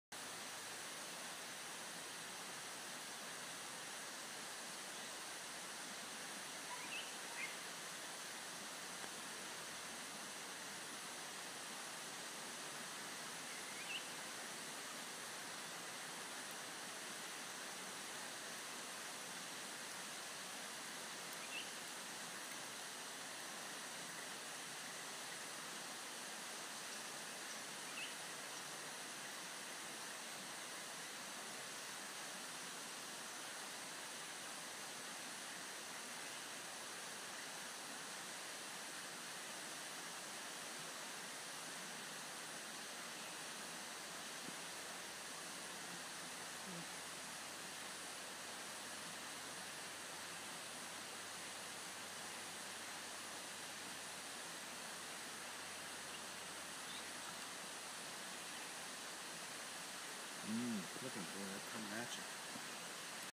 Bird photography in Thailand by Wild Bird Eco Tour
Buffy Fish-Owl ,Uncommon resident.At Khoa Yai National Park ,Thailand.